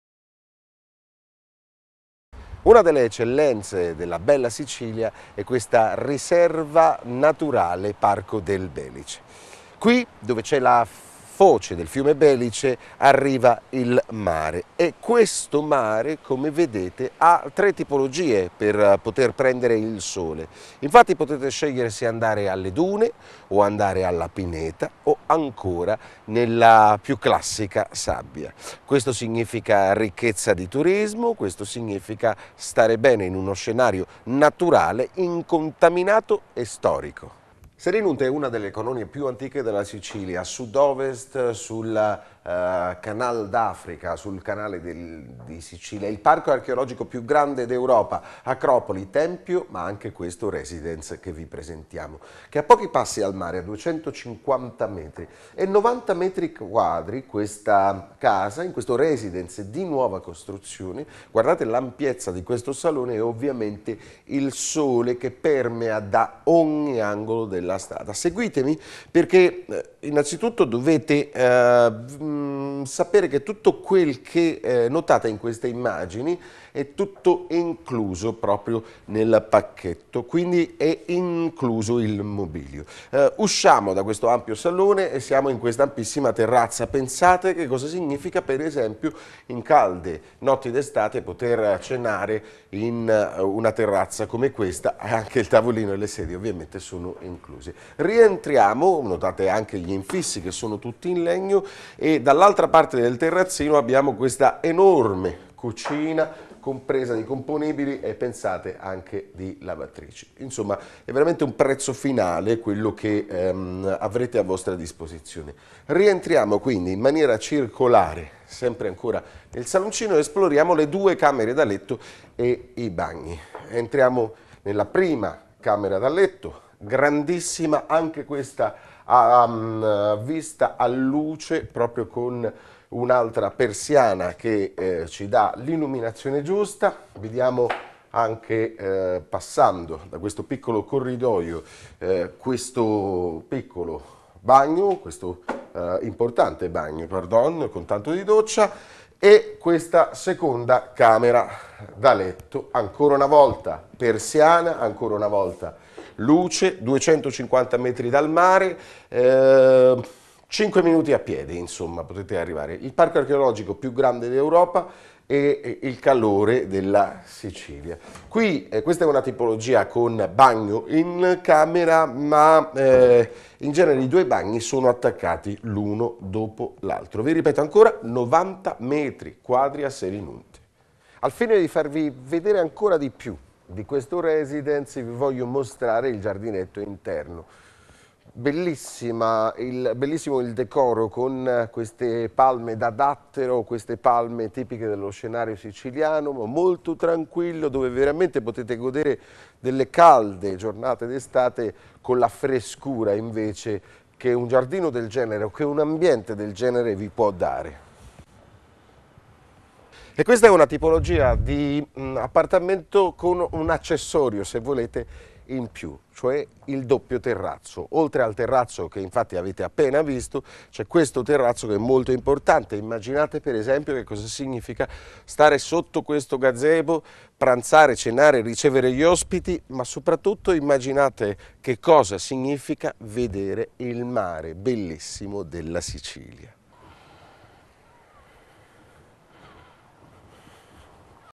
Appartamento Selinunte (Italiano) - R'ESTATE IN SICILY
Selinunte, luogo pieno di storia, di cultura e...MARE! Il Parco archeologico più grande d'Europa, un appartamento in un Condominio privato, a pochi passi dalla spiaggia!